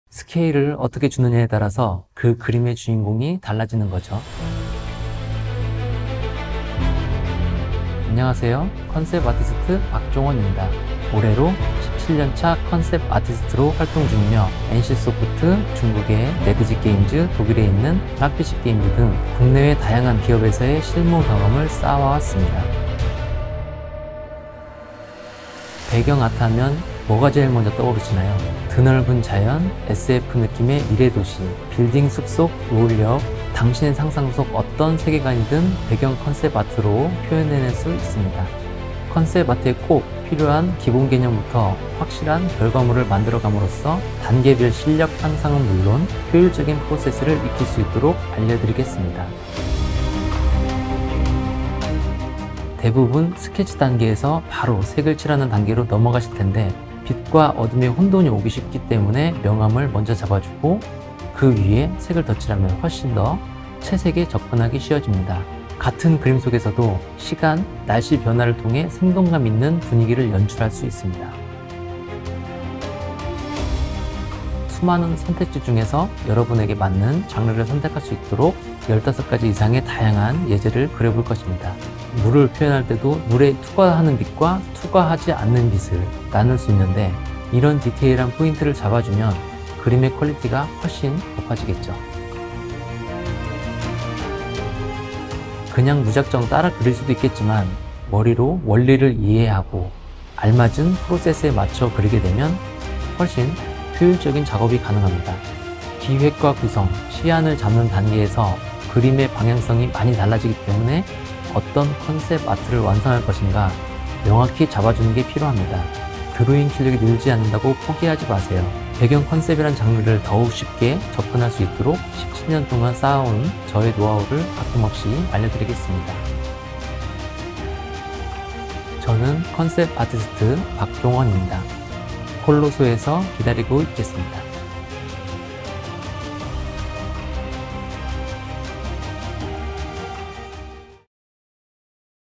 15가지 이상의 다양한 컨셉별 배경 배우기!ㅣ컨셉아티스트 박종원
어딘가 부족한 나의 포트폴리오?..

회사가 원하는 포폴은 하나의 컨셉 아트가 아닌
다양한 느낌의 컨셉 드로잉을 원하죠!
NC소프트, 중국의 넷이즈 게임즈 등 글로벌 기업의
배경 컨셉아트를 담당한 박종원 컨셉아티스트의
16년 노하우를 알려드릴게요.
이번 클래스를 통해서 기본적인
배경 컨셉아트의 원리를 이해하고,
실무에서 활용되는 다양한 컨셉의
배경아트를 완성해갈 수 있습니다.